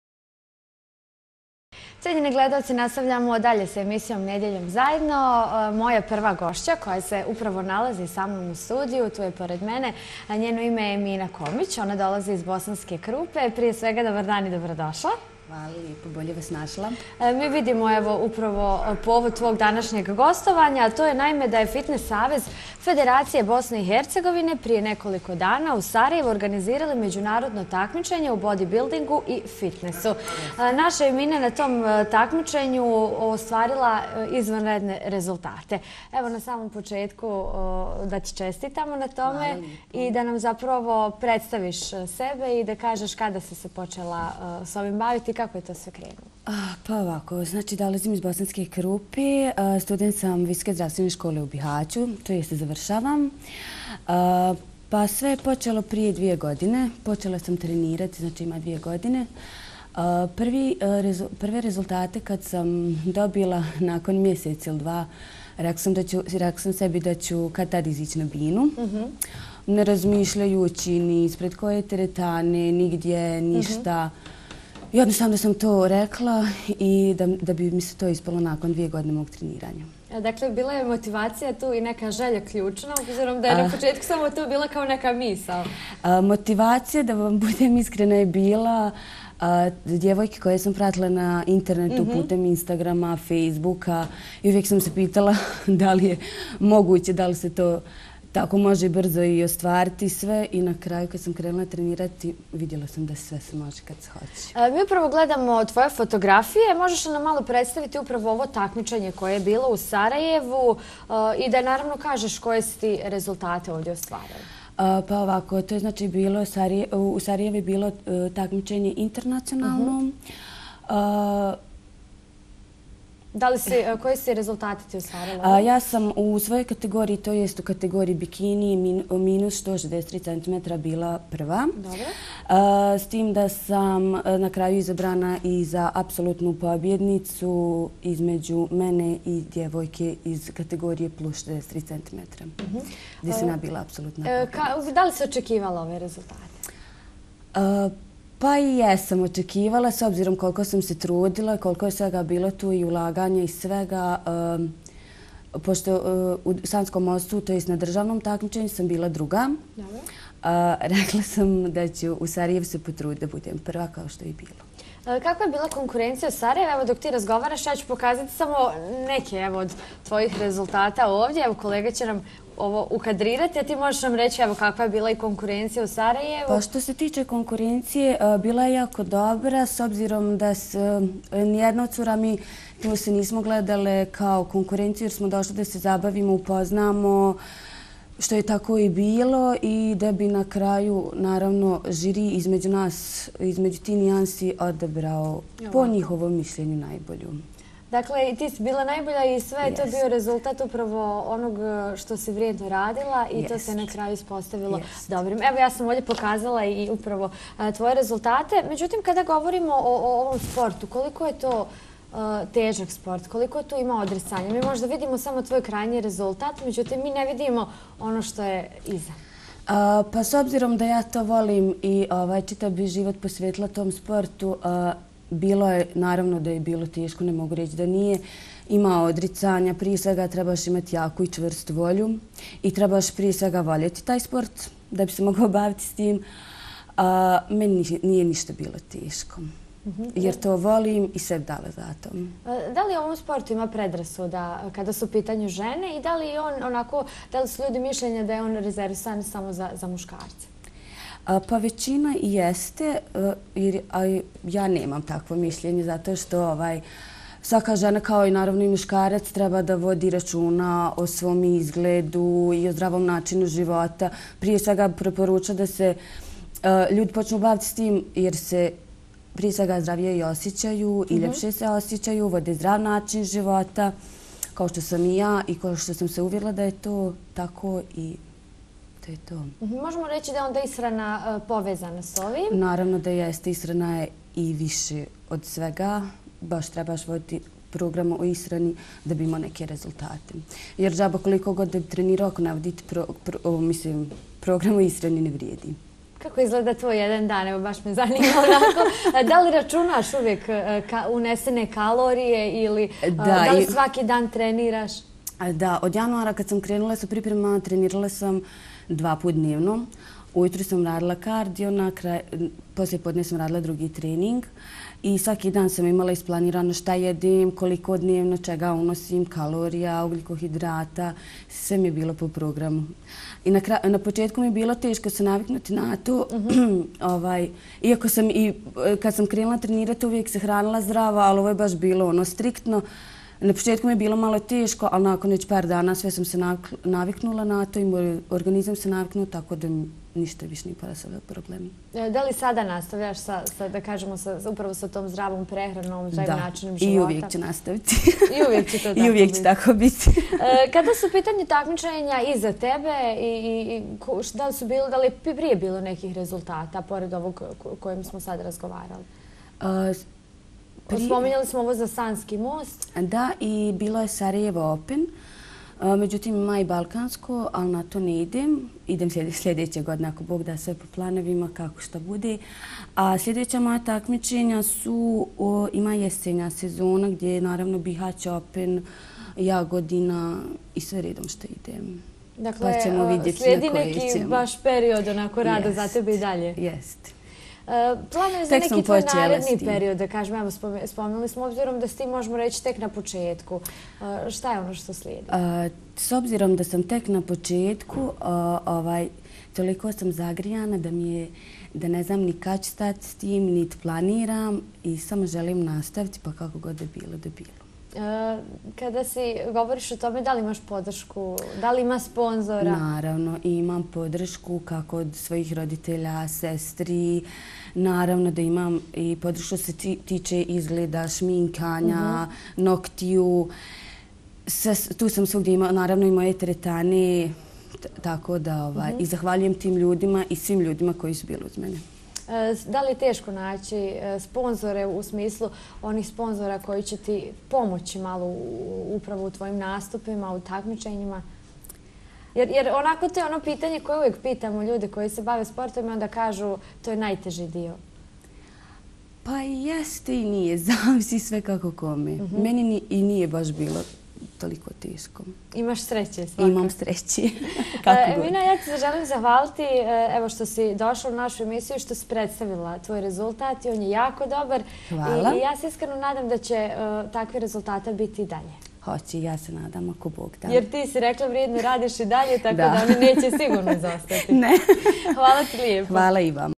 Krupljanka Emina Komić apsolutna pobjednica “Sarajevo Opena”
Bodybuilding takmičenje: Krupljanka Emina Komić apsolutna pobjednica “Sarajevo Opena”